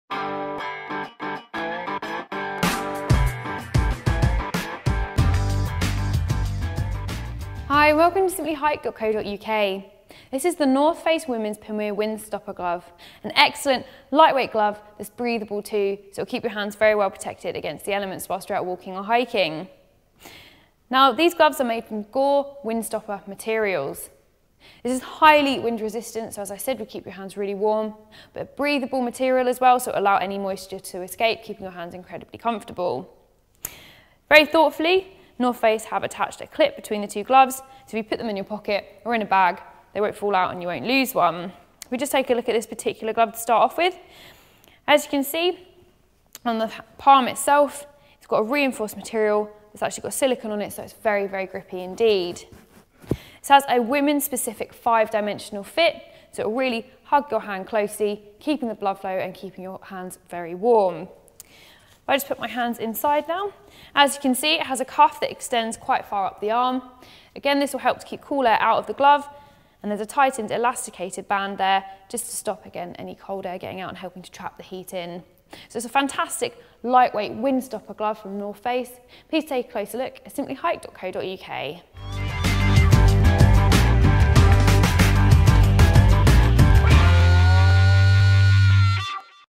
The North Face Womens Pamir Windstopper Glove
The North Face Womens Pamir Windstopper Glove is a womens specific glove that will keep your hands well insulated.  Made from Gore Windstopper material that will keep the wind chill out whilst allowing excess moisture to escape as it is breathable.